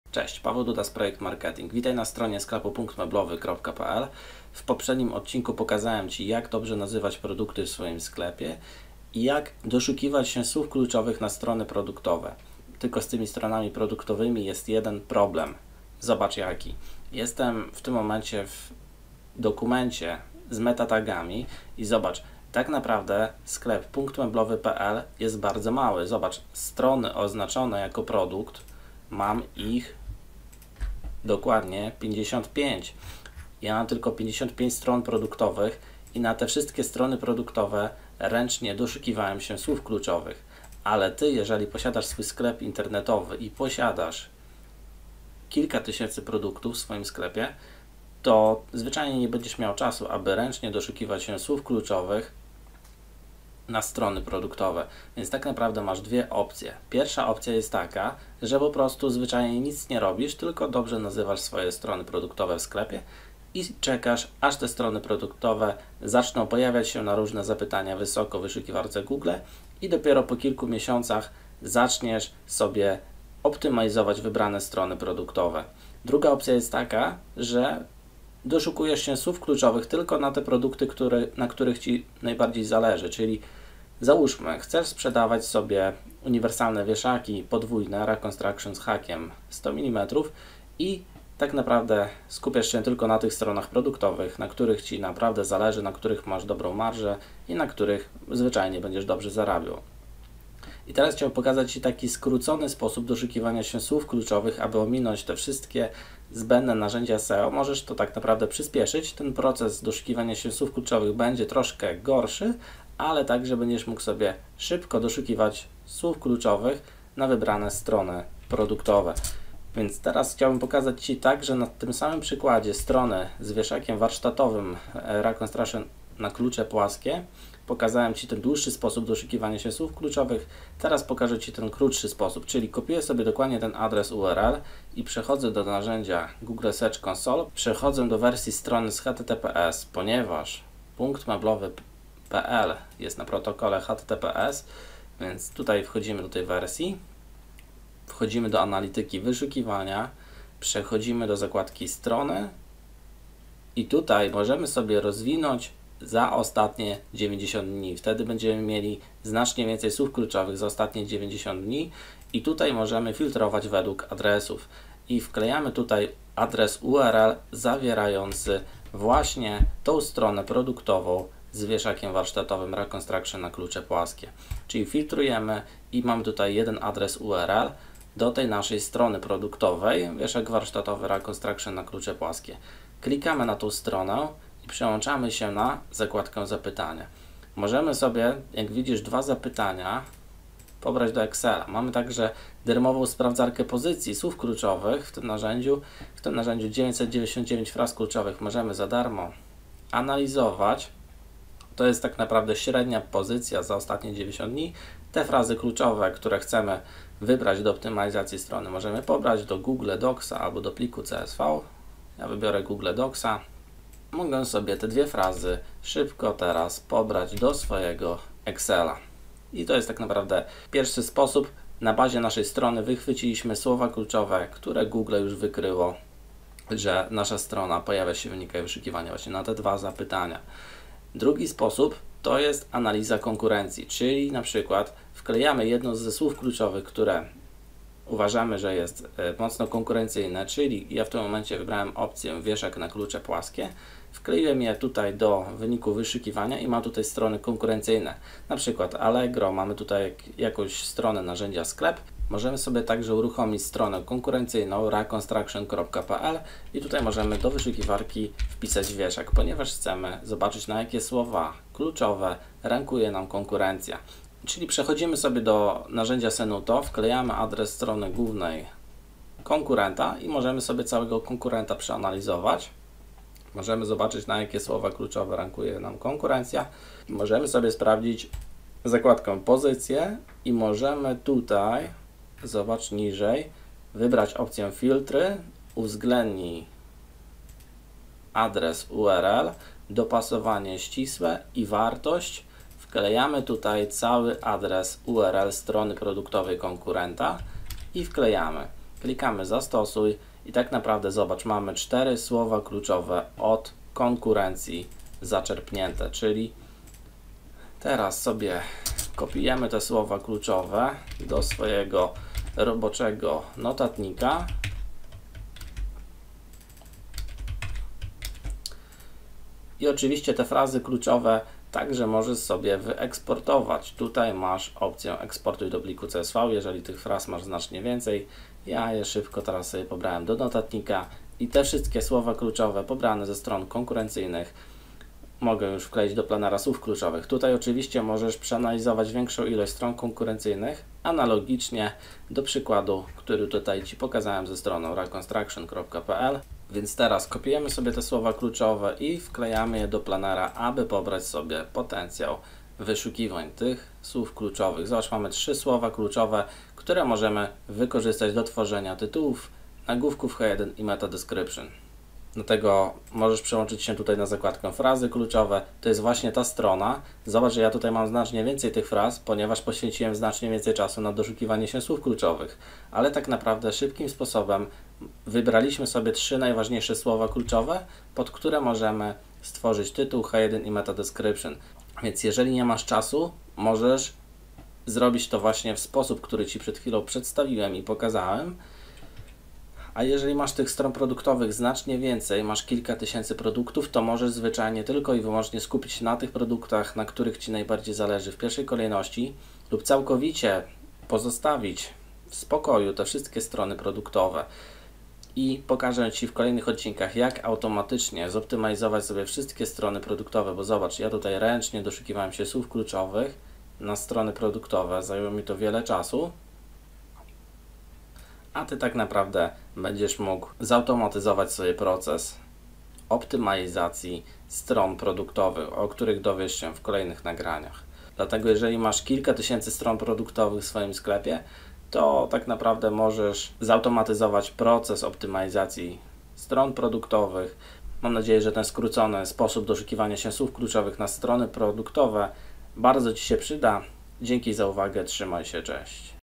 Jak znaleźć frazy pod SEO dla stron produktowych na bazie strony i konkurencji
Jak krok po kroku znaleźć dobre słowa kluczowe pod pozycjonowanie stron produktowych na bazie własnej strony, narzędzia Google Search Console i analizy konkurencji.